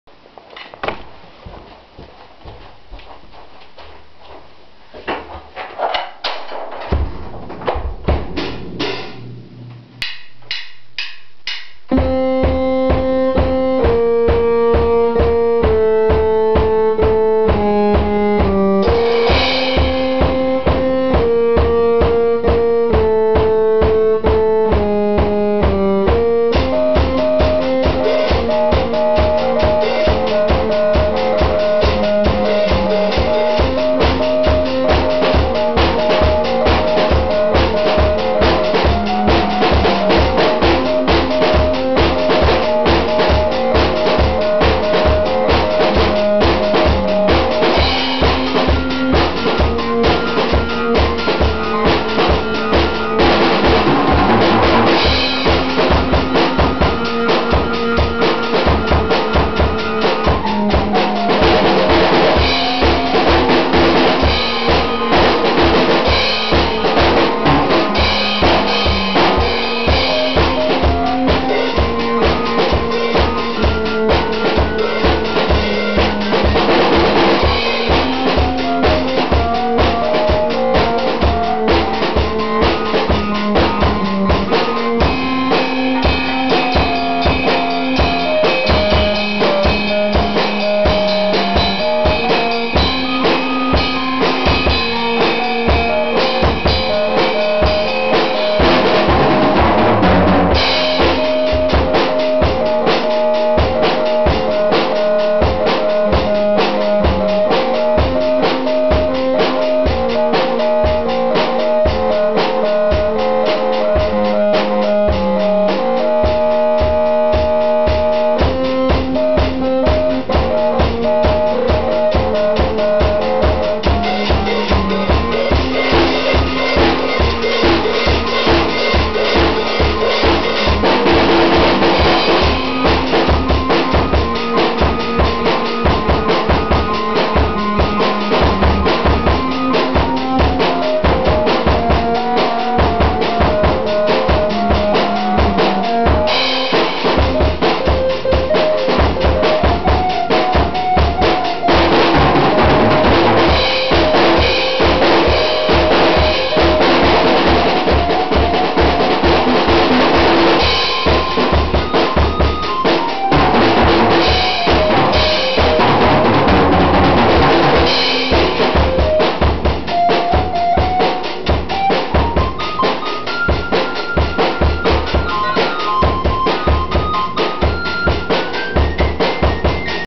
spence-colby
me and colby rippin up a sick jam  4
ashley(ilu)  and brianna